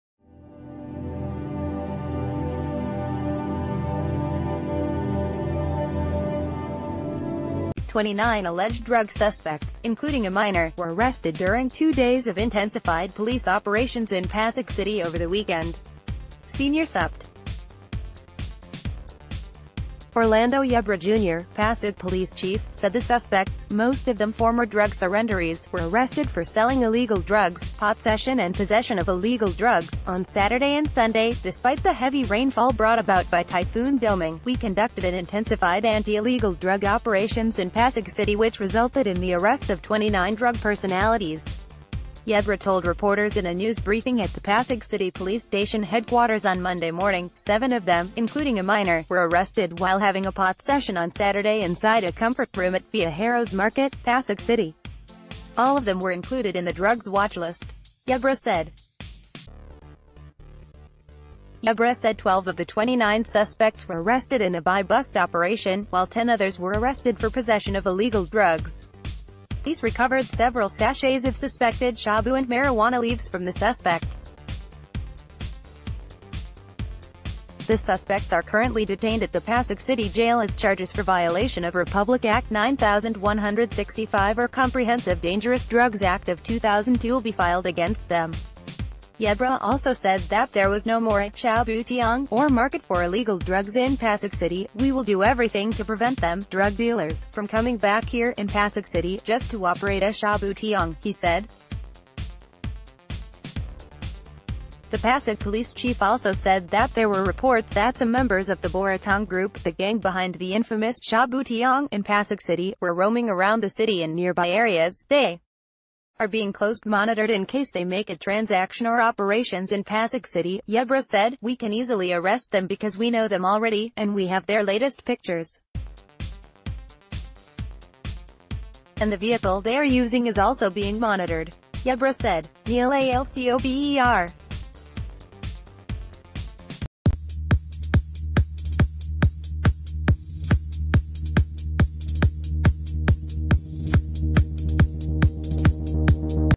Breaking News | 29 nabbed in Pasig weekend anti-drug operations
TWENTY-nine alleged drug suspects, including a minor, were arrested during two days of intensified police operations in Pasig City over the weekend. Senior Supt. Orlando Yebra Jr., Pasig police chief, said the suspects — most of them former drug surrenderees — were arrested for selling illegal drugs, pot session and possession of illegal drugs.“On Saturday and Sunday, despite the heavy rainfall brought about by Typhoon Domeng, we conducted an intensified anti-illegal drug operations in Pasig Cit...